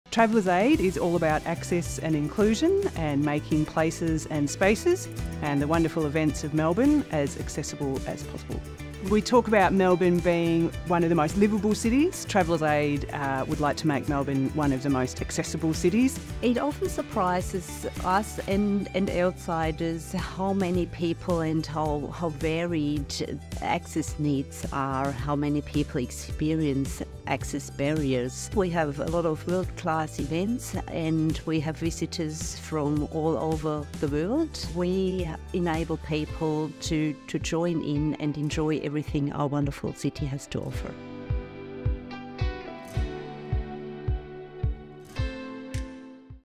Travellers Aid honoured with Melbourne Award for Access and Inclusion 2024
Being acknowledged at the MelbourneAwards for our contribution to make Melbourne not only one of the world’s most liveable cities but certainly one of the world’s more fair, inclusive and accessible cities is quite an honour. Thank you to our volunteers and staff for your commitment to enabling everyone to participate in their communities.